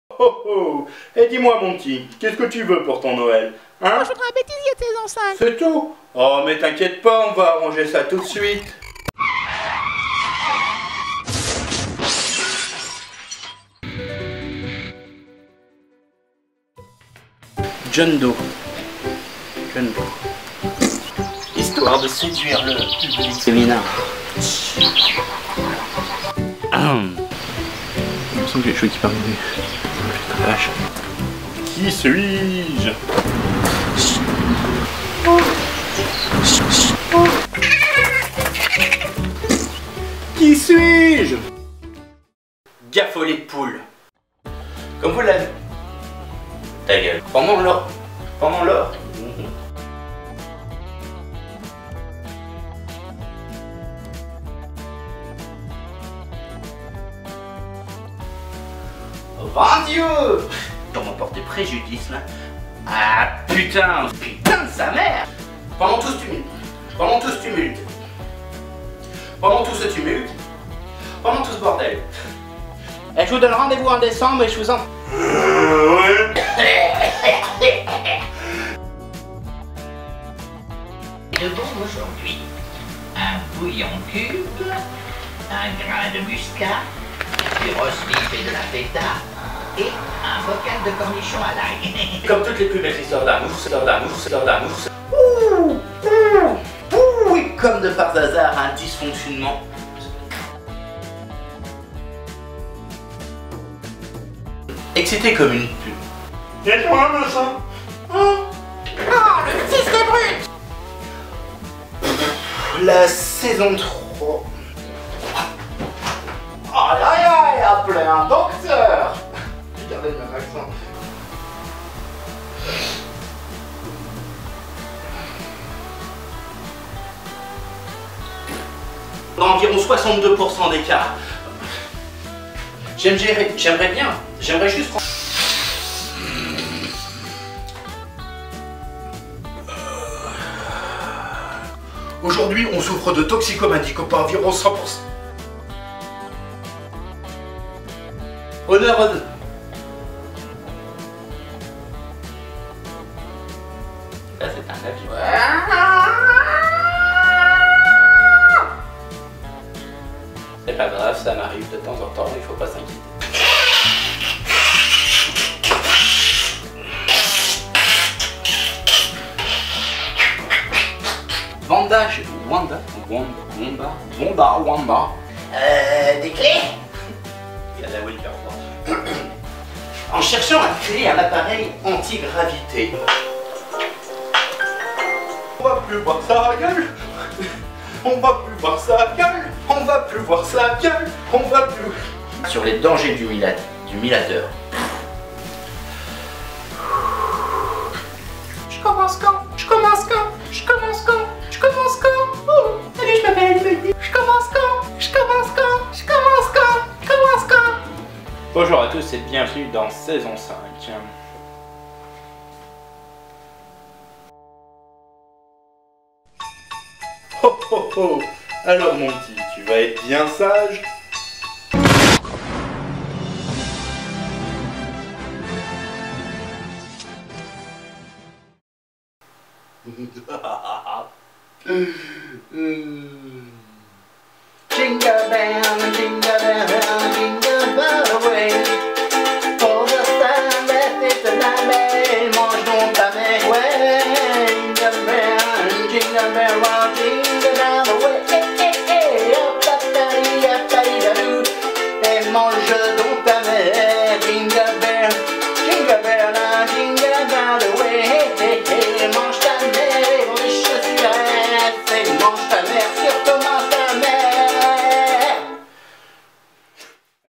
Saison 5 -#01- HS spécial noël (bêtisier)
Voici un bêtisier rien que pour vous bande de pti salopiot, joyeux noël a tous  HOHOHOOOOOOOO